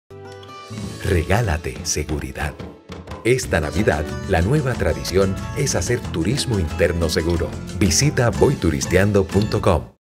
Celebrate by exploring Puerto Rico's magic!
The Puerto Rico Tourism Co. is urging local residents to create a new tradition this holiday season by enjoying — and exploring — everything the island has to offer! for more information.